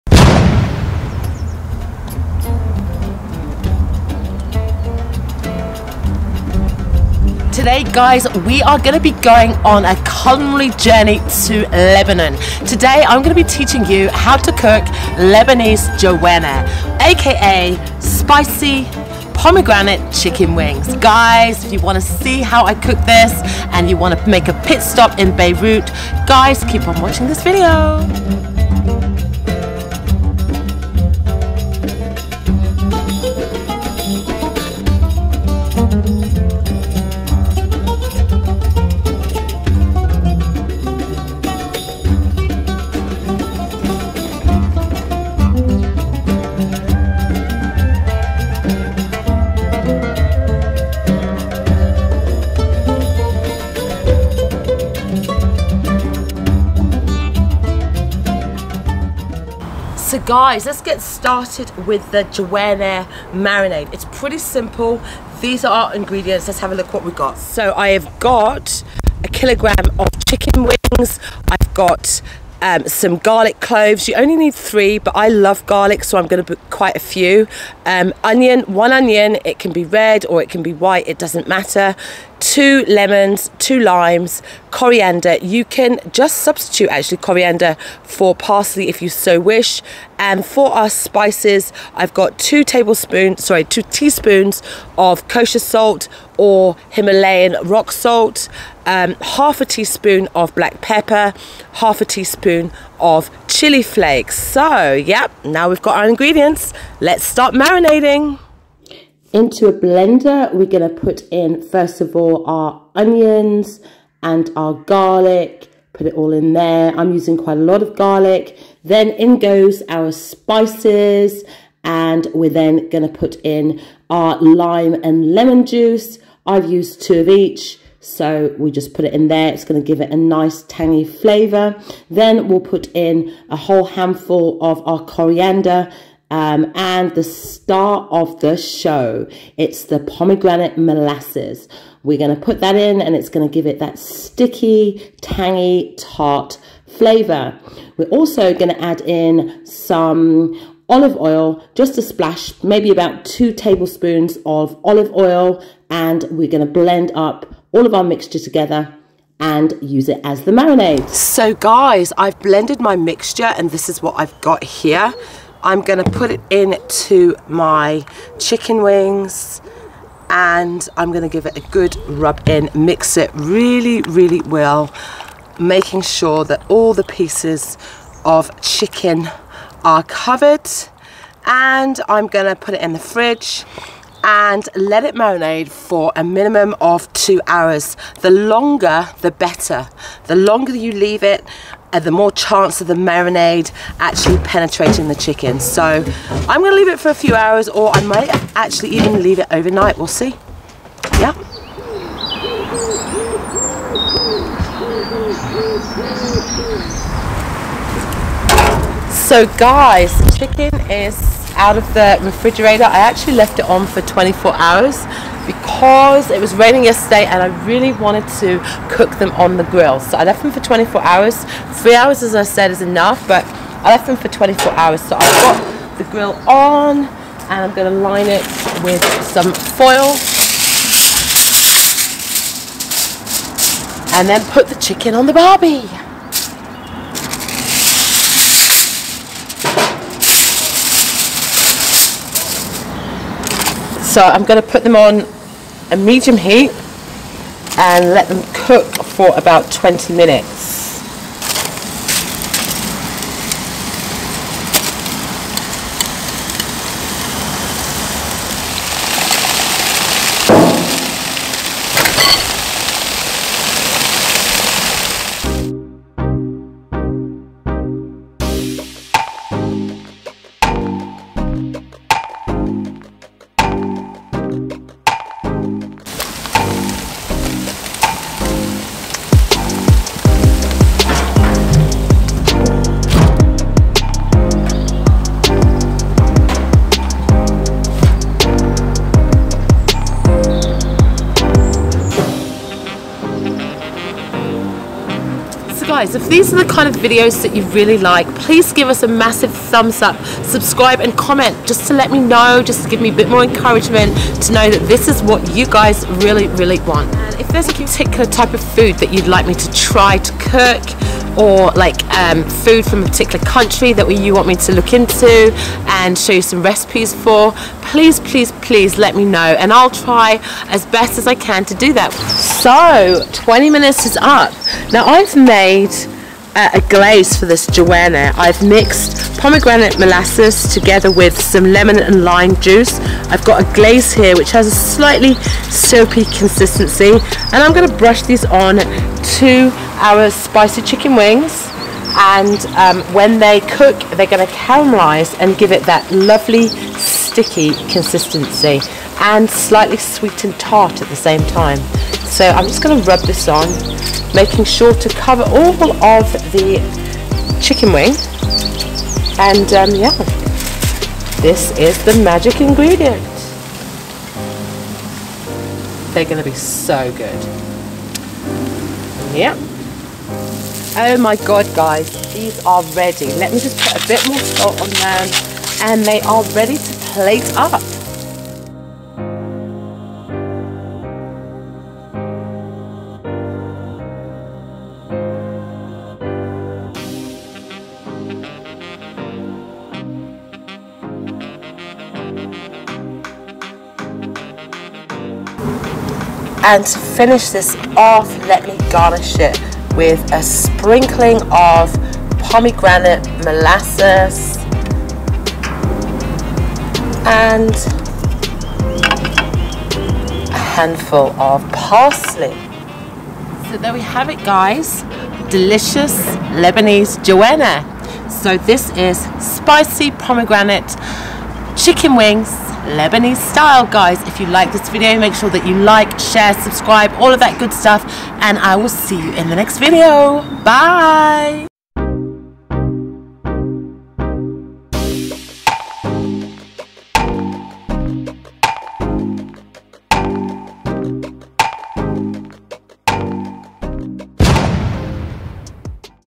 🇱🇧BBQ Sticky Pomegranate Chicken - Lebanese Jawaneh - جوانح بدبس الرمان | Gur Eats Middle East 🐫
Hi Guys,

In this video I cook : 🇱🇧BBQ Sticky Pomegranate Chicken - Lebanese Jawaneh - جوانح بدبس الرمان .
Jawaneh (or Jwenih) are chicken wings cooked with coriander, garlic and lemon, served as mezze.

Lebanese dishes are heavily influenced by the multiple civilisations that have existed within the region, which has accumulated together to form the modern Lebanese cuisine we know today. Using fresh, flavourful ingredients and spices, Lebanese cuisine combines Turkish, Arab, and French cooking styles.

The ingredients for these delicious chicken wings:

1kg of Chicken Wings, or other chicken pieces
1 Onion
4 Lemons and Lime
3 Cloves of Garlic
A bunch of Coriander or Parsley
3 Tbsp Pomegranate Molasses
2 Tbsp Olive Oil
2 Tsp Himalayan Rock Salt or Kosher Salt
0.5 Tsp Black Pepper
0.5 Tsp Chilli Flakes

🧡TRUTHFULLY APPRECIATE YOUR SUPPORT🧡